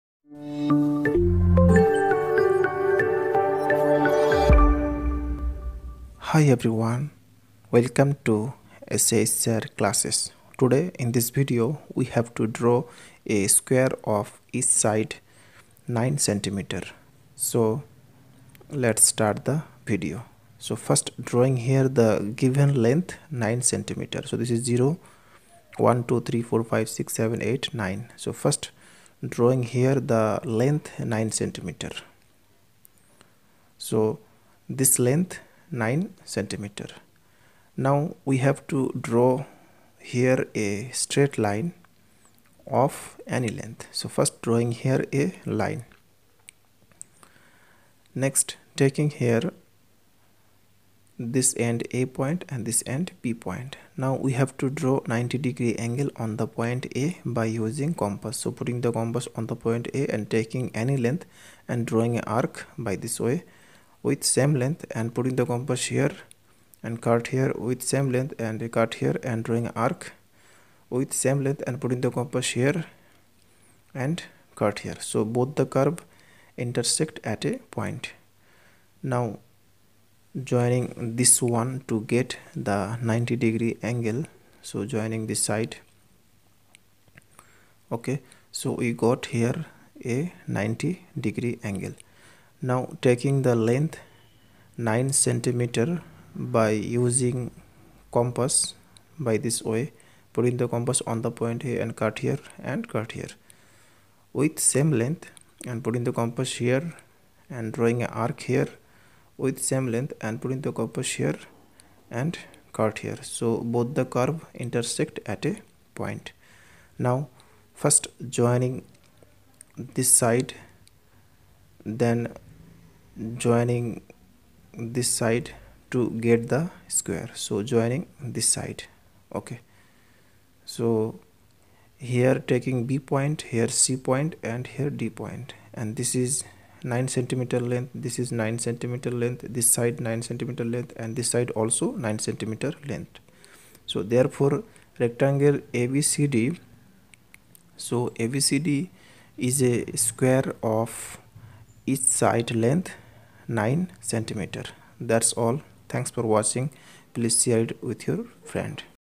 How to draw a square of each side 9 cm.shsirclasses.
In Easy Way how to draw a square of each side 9 cm.shsirclasses. Geometrical Construction.